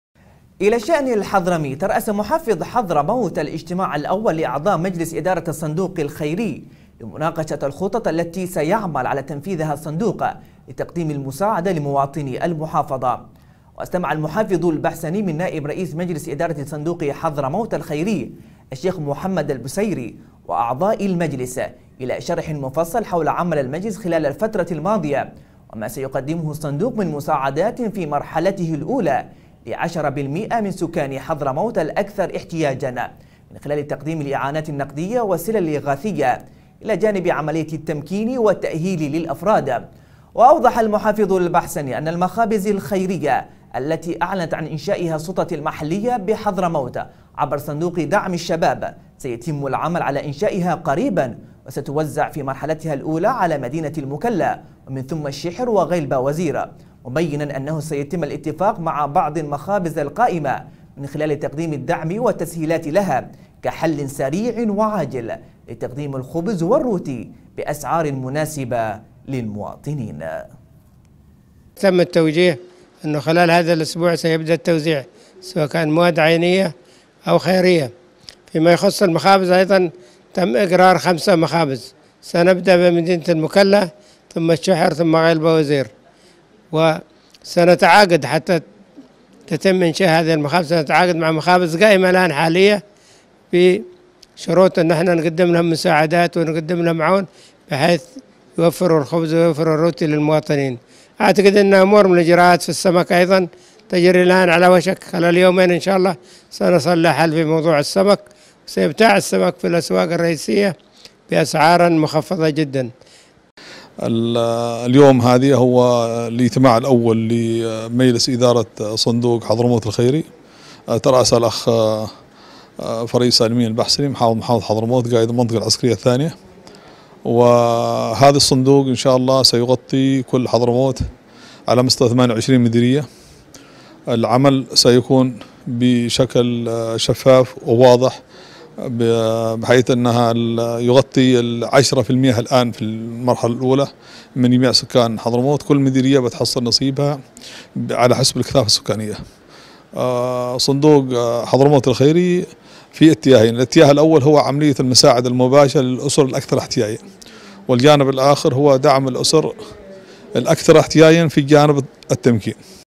خبر | المحافظ البحسني يترأس الاجتماع الأول لأعضاء مجلس إدارة صندوق حضرموت الخيري
ترأس محافظ حضرموت الاجتماع الأول لأعضاء مجلس إدارة الصندوق الخيري لمناقشة الخطط التي سيعمل على تنفيذها الصندوق لتقديم المساعدة لمواطني المحافظة.